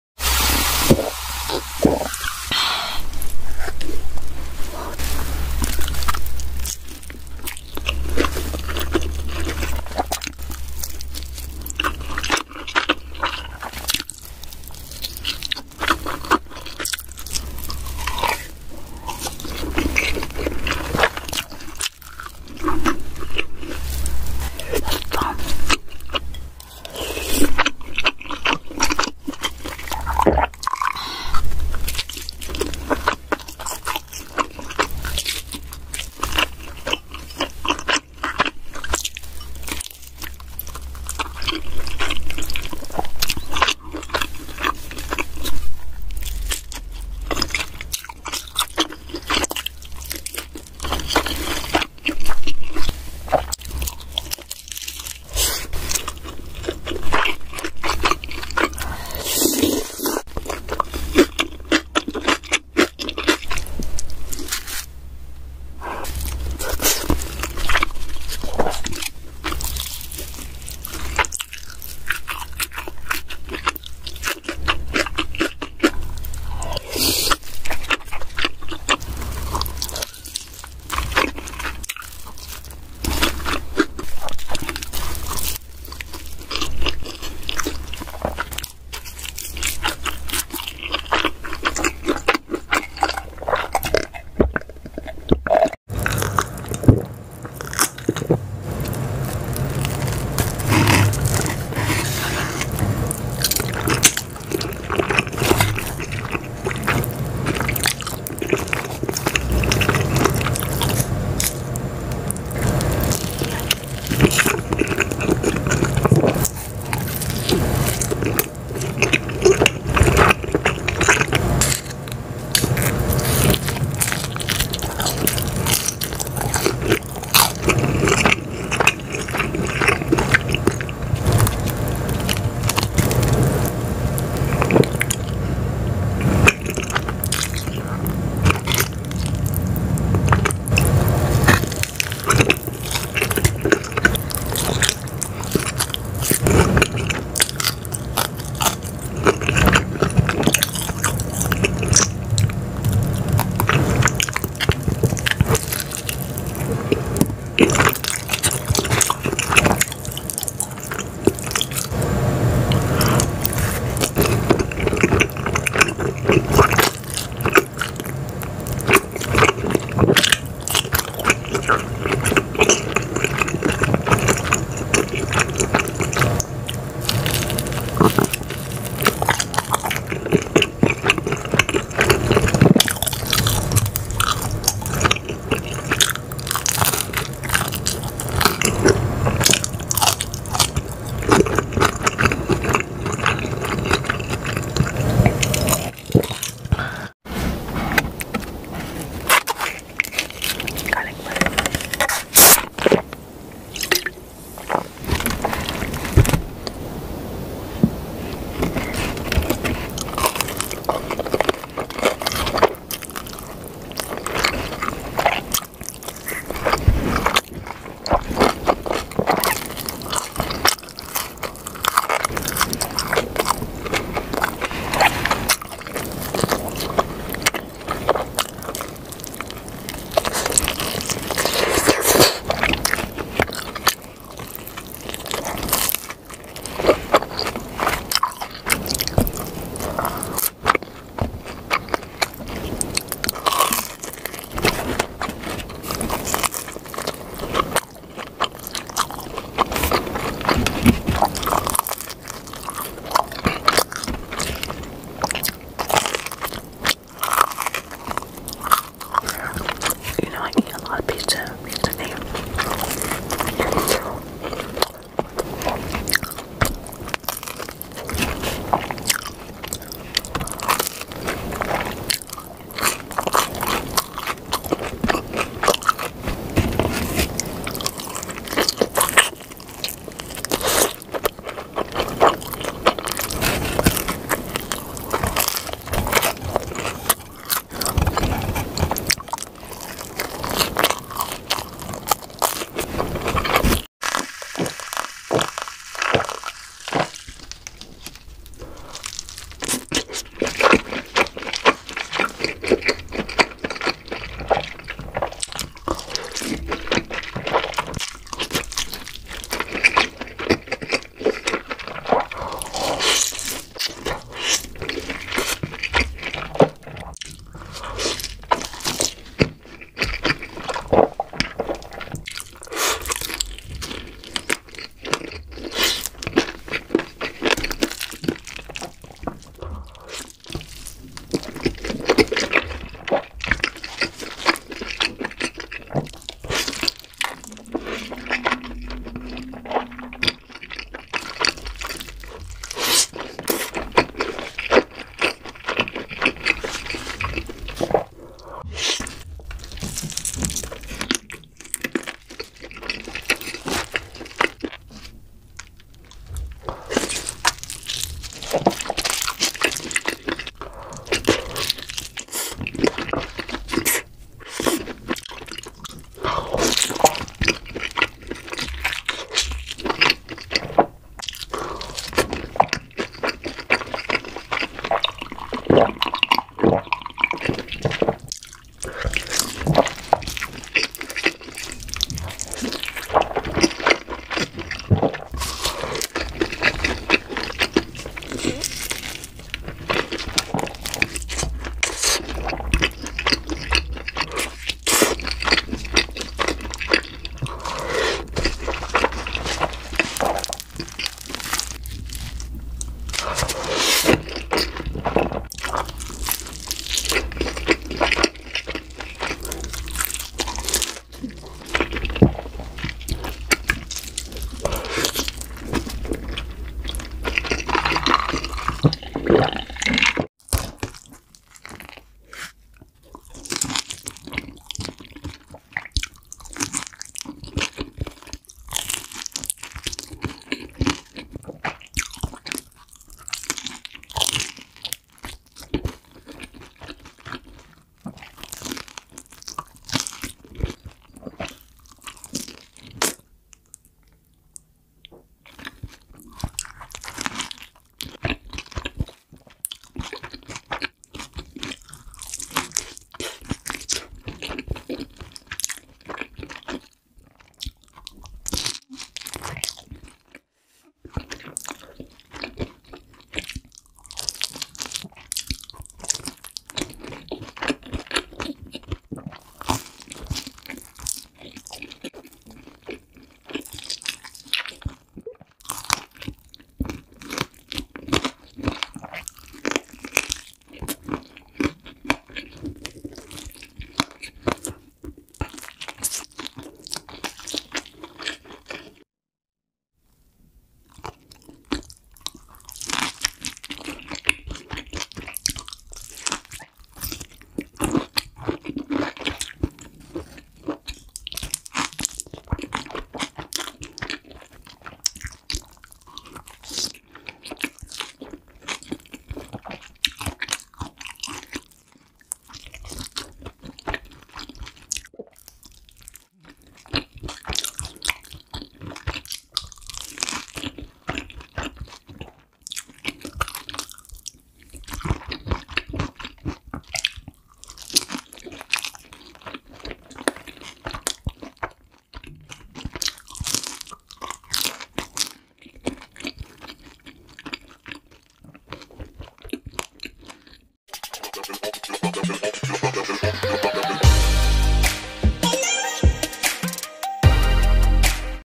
ASMR MUKBANGERS EATING Cheesy Pizza Mukbang🍕
Im editing and making mukbang videos
I make an effort to edit this compilation video to make it more satisfying to watch because I love watching Mukbang videos too ✨🤗🤍

Any Mukbang Asmr Request Suggestions? Just comment